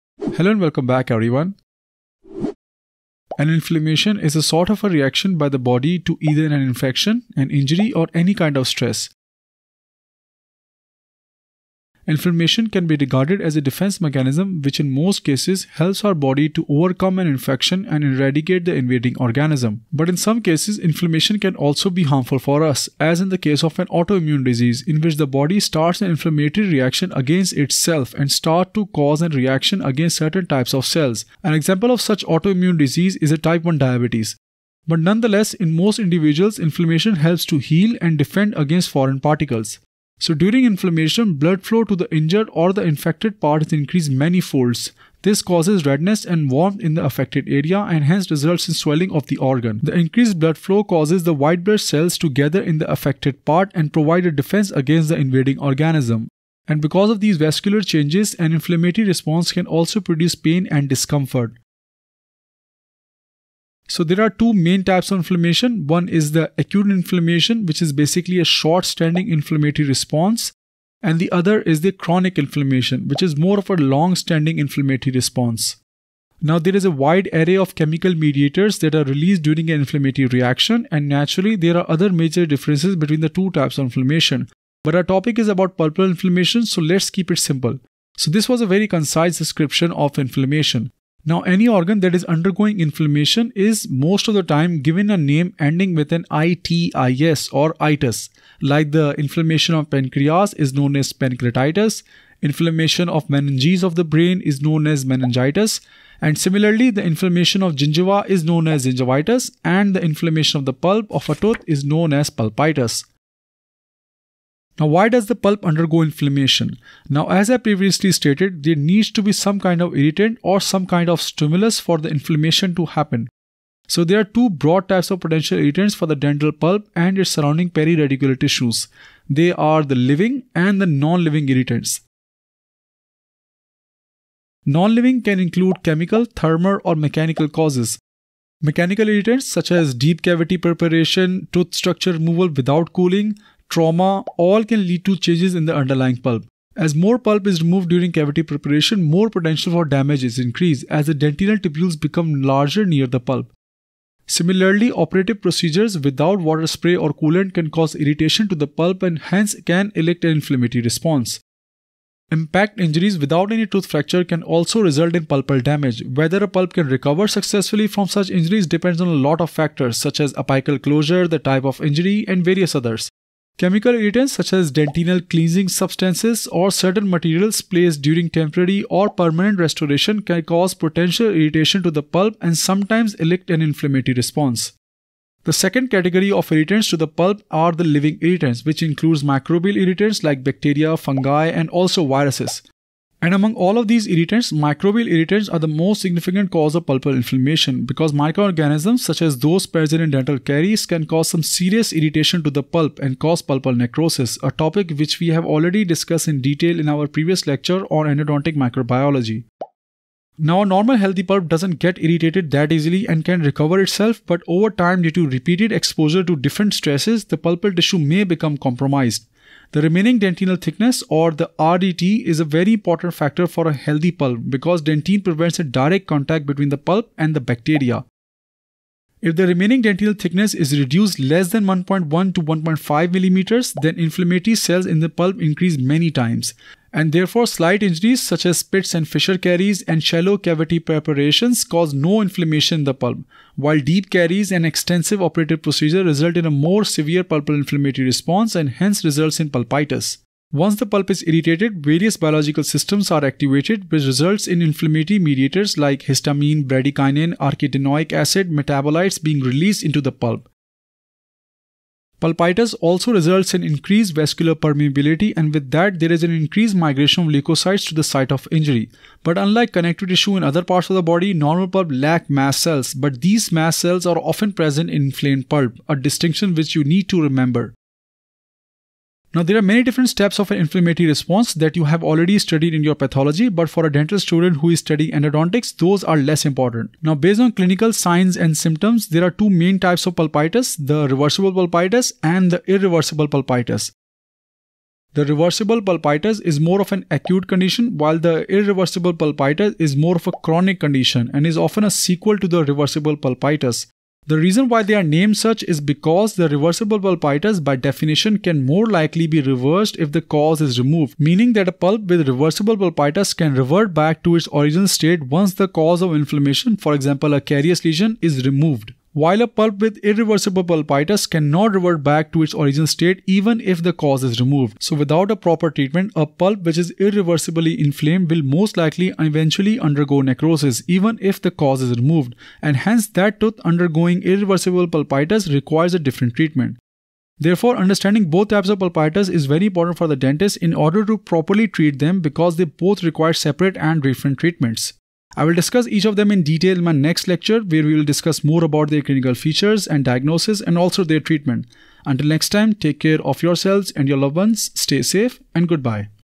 What is Pulpitis? | Endodontics
In today's lecture, I have tried to explain What is Pulpitis? and what are the different types of Pulpitis?
Hello everyone. I hope all of you are doing great and taking care of yourselves.
So What is Pulpitis?
- Pulpitis in simple words is the inflammation of the pulp.
- Inflation is a process by which our body defends itself and heals itself.
- But sometimes inflammation can also be harmful for us. As in the case of an autoimmune diseases.
- Inflammation causes redness and warmth in the affected organ and can also cause pain and discomfort.
- Hence pulpitis is a process that occurs within the pulp when there is an external stimuli, like bacterial attack or mechanical trauma to the the pulp.
- This external stress causes the pulp to get inflamed and hence undergo pulpitis.
- How many types of pulpitis are there? There are two types of pulpitis. Reversible pulpitis and Irreversible pulpitis.
- Reversible pulpitis is an acute inflammatory condition which can revert back to it's original state once the cause of inflammation is removed.
- Irreversible pulpitis is a chronic condition which most of the time, cannot revert back even if the cause is removed.

Comment down any other topic you want me to make video on.

Leave your feedback in the comment section below.

Chapters
What is inflammation? 00:00
What is Pulpitis?  01:49
Ending 05:36